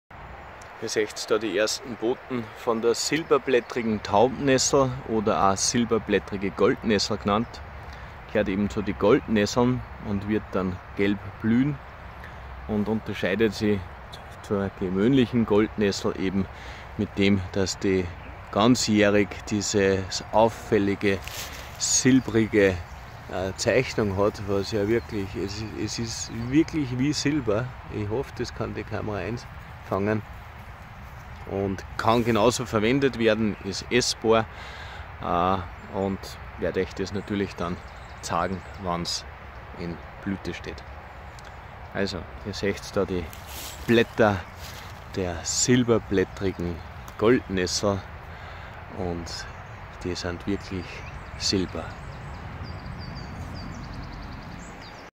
Silberblättrige Taubnessel - Erste Boten - 10.04.18 (Lamium argentatum) - Wildpflanzen-Bestimmung
Die Silberblättrige Taubnessel (Lamium argentatum) ist eine Unterart der Goldnessel (Gelbe Taubnessel), bei der alle Blätter das ganze Jahr über silbrig gezeichnet sind, während bei der Gewöhnlichen Goldnessel die Blätter höchstens im Winter zum Teil nur schwach gezeichnet sind. Außerdem hat die Silberblättrige Taubnessel eine etwas längere Blütenoberlippe. Sie kommt in West- und Mitteleuropa vor.

Büchertipps für Wildkräuter- & Pflanzenbestimmung
- Essbare Wildpflanzen - 200 Arten bestimmen und verwenden.
- Pflanzliche Notnahrung: Survivalwissen für Extremsituationen.
- Enzyklopädie essbare Wildpflanzen (die "Bibel" der Wildpflanzen :-))